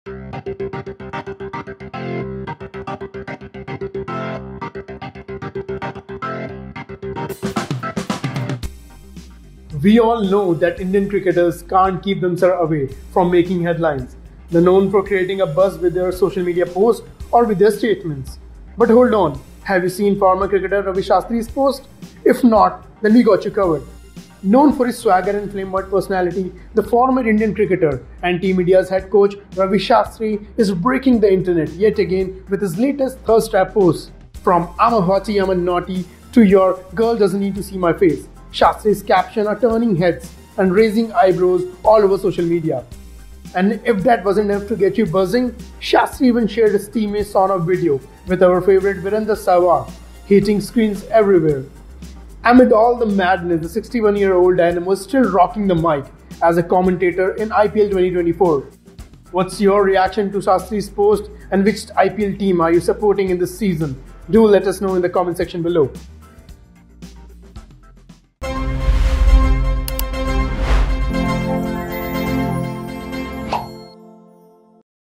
“I am Hottie, I am Naughty...” Ravi Shastri Breaks The Internet With His ‘Thirst Trap’ Posts | Watch
“Iam Hottie, Iam Naughty...” Ravi Shastri Breaks The Internet With His ‘Thirst Trap’ Posts | Watch

We all know that Indian cricketers can’t keep themselves away from making headlines. They are known for creating a buzz with their social media post or with their statement. But Hold On, have you seen Former Cricketer Ravi Shastri’s Post. If not, then we got you covered. Known for his swagger and style, the iconic Indian Cricketer and Team India’s Head Coach, Ravi Shastri, is breaking the internet yet again with his latest 'Thirst Trap' Posts. From 'I am Hottie, I am Naughty' to 'Your girl doesn't need to see my face', Shastri's captions are turning heads and raising eyebrows all over social media. But wait, there's more! These viral posts are part of an epic campaign for the travel giant MakeMyTrip. proving that Shastri's game is on point both on and off the field. And if that wasn't enough to get you buzzing, Shastri even shared a steamy sauna video.  with our favorite Virender Sehwag, heating screens everywhere! Amidst all the madness, the 61-year-old dynamo is still rocking the mic as a commentator in the Indian Premier League (IPL) 2024.